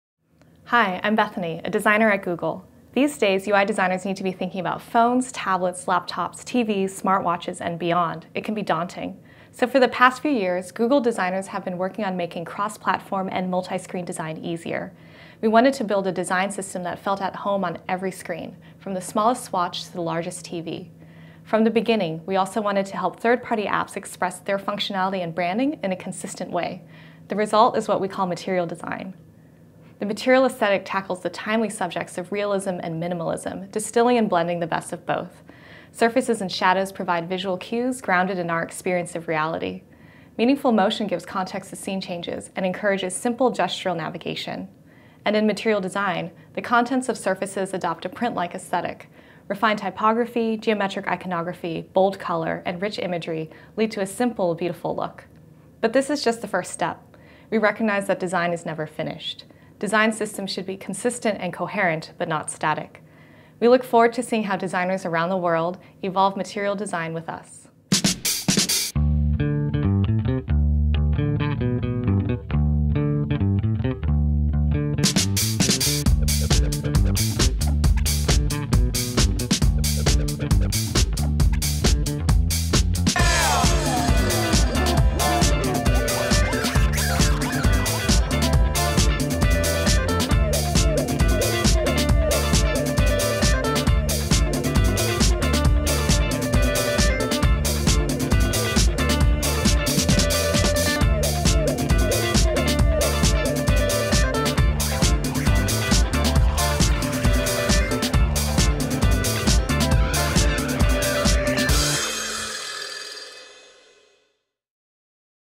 DesignBytes: Intro To Material Design
These days, UI designers need to be thinking about phones, tablets, laptops, TVs, smartwatches, and beyond. In this DesignByte we talk about how Google designers have been working on making cross-platform and multi-screen design easier. We wanted to build a design system that felt at home on every screen, from the smallest watch to the largest TV.